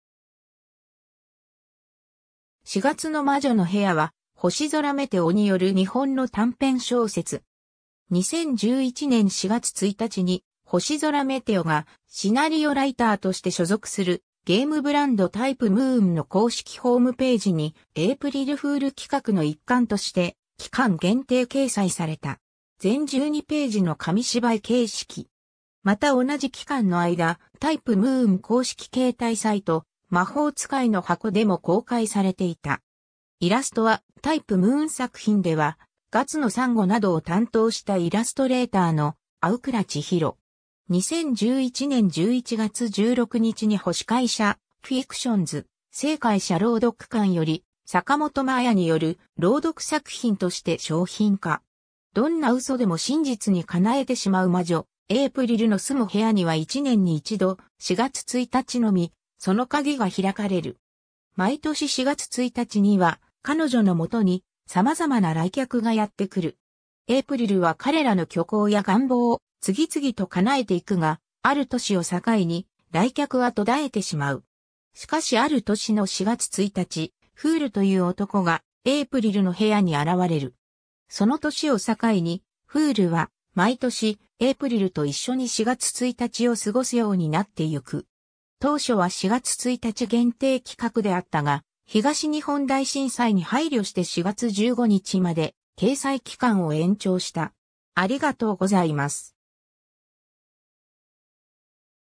四月の魔女の部屋
TYPE-MOON
『四月の魔女の部屋』（しがつのまじょのへや）は、星空めておによる日本の短編小説。
 2011年4月1日に星空めておがシナリオライターとして所属するゲームブランド・TYPE-MOONの公式ホームページに、エイプリルフール企画の一環として期間限定掲載された。
全12ページの紙芝居形式。
また同じ期間の間、TYPE-MOON公式携帯サイト「まほうつかいの箱」でも公開されていた。
 イラストはTYPE-MOON作品では『月の珊瑚』などを担当したイラストレーターの逢倉千尋。
 2011年11月16日に星海社FICTIONS・星海社朗読館より、坂本真綾による朗読作品として商品化。
 どんな嘘でも真実にかなえてしまう魔女・エイプリルの住む部屋には1年に1度、4月1日のみその鍵が開かれる。
毎年4月1日には彼女の元に様々な来客がやって来る。
エイプリルは彼らの虚構や願望を次々と叶えていくが、ある年を境に来客は途絶えてしまう。
しかしある年の4月1日、フールという男がエイプリルの部屋に現れる。
その年を境にフールは毎年、エイプリルと一緒に4月1日を過ごすようになってゆく。
 ^ 当初は4月1日限定企画であったが、東日本大震災に配慮して4月15日まで掲載期間を延長した。